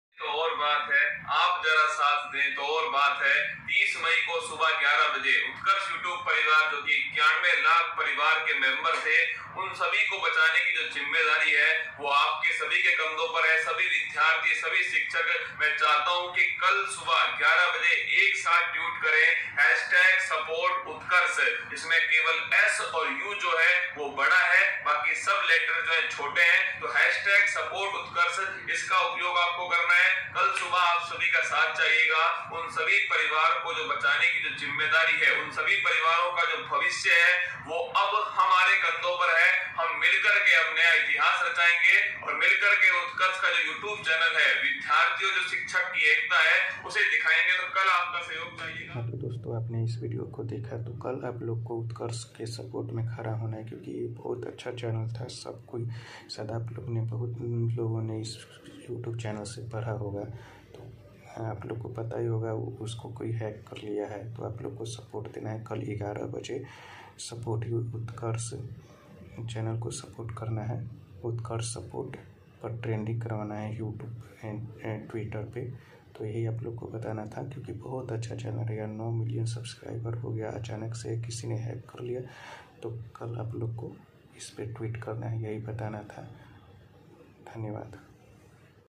utkarsh classes jodhpur ko कल aaplog को support krna hai|| 11 bje twitter champion for utkarsh class
पे कल 11 बजे ट्विटर campain करना है